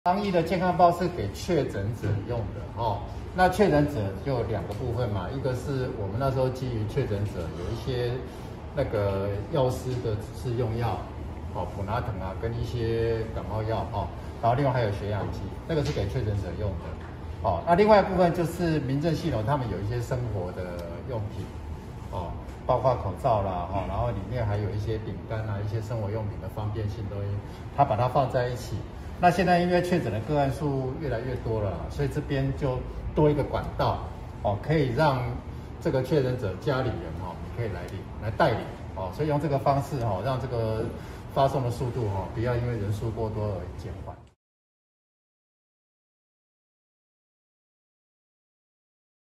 彰化+1169！代領「防疫健康包」更便捷 戶政所設便利站
彰化縣今天新增1169例本土確診，因應即將進入疫情高峰期，為了讓確診者和居家檢疫者安心治療和隔離，彰化縣政府新設「戶政防疫便利站」，當事人可請未確診的親友代領防疫健康包，以免因久候專人送達造成隔離期的生活不便。防疫健康包內有藥師指示用藥，例如普拿疼和感冒藥，以及血氧機，還有隔離期間日常生活用品，口罩